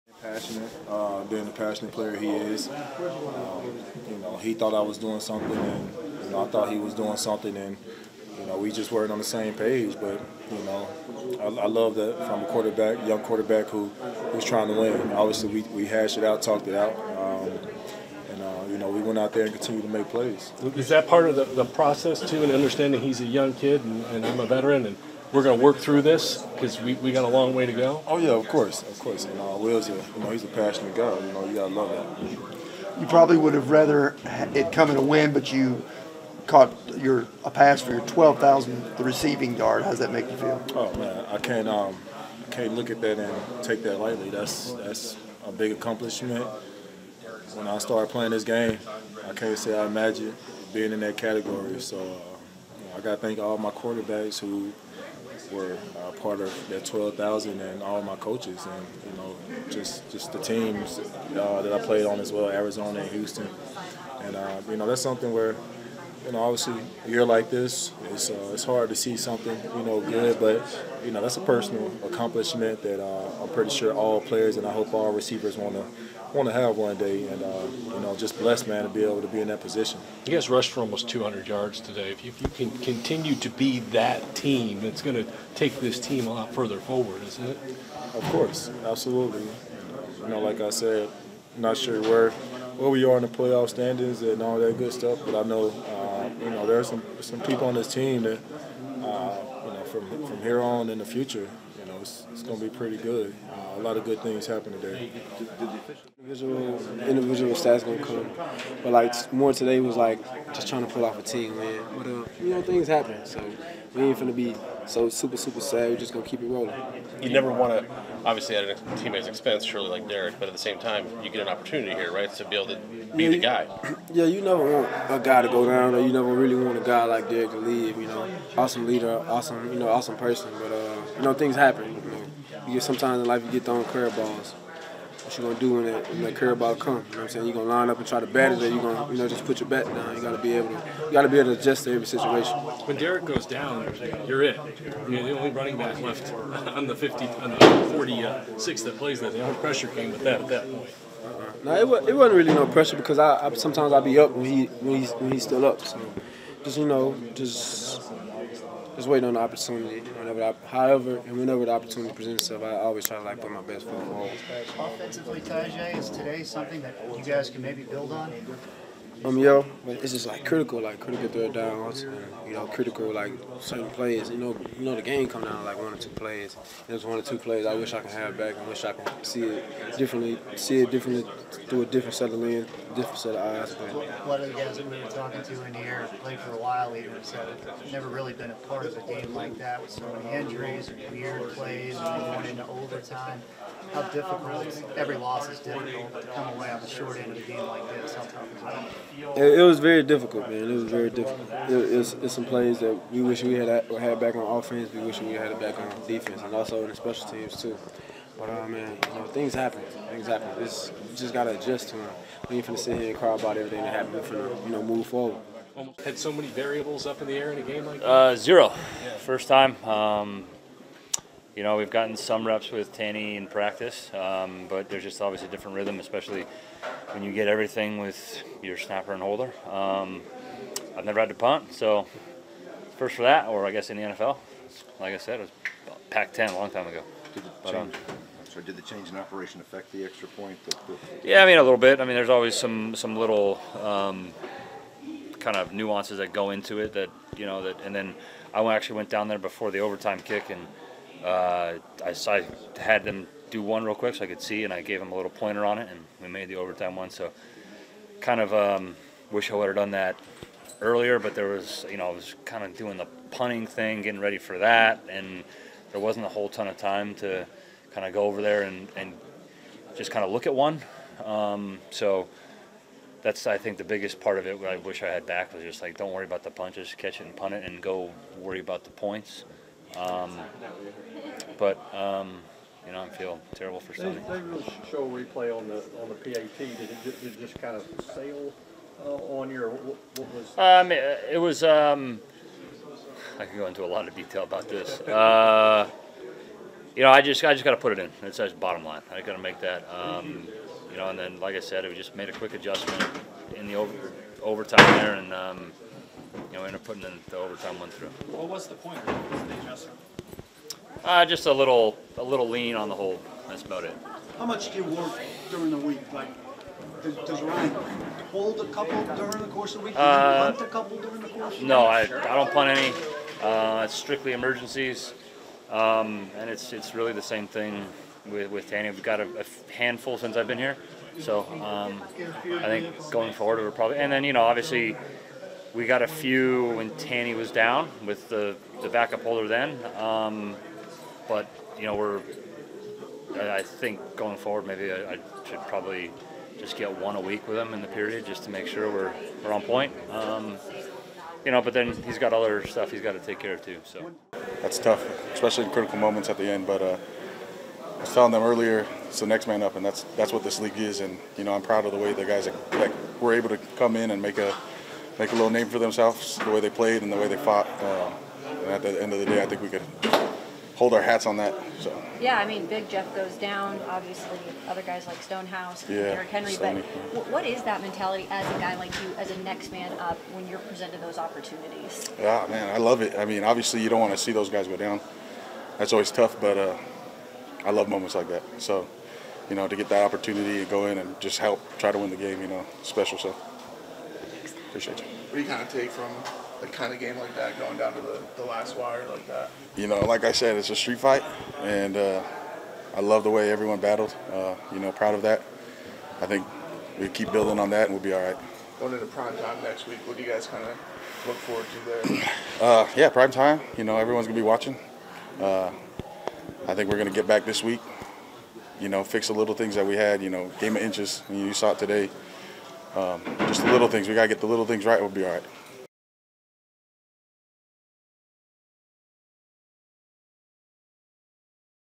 Adjust in Any Situation | Media Availability
00:00 - WR DeAndre Hopkins
01:50 - RB Tyjae Spears
03:37 - K Nick Folk
05:55 - DL Kyle Peko

Titans WR DeAndre Hopkins, RB Tyjae Spears, K Nick Folk and DL Kyle Peko address reporters during a Postgame Press Conference after the Week 13 game against the Colts at Nissan Stadium.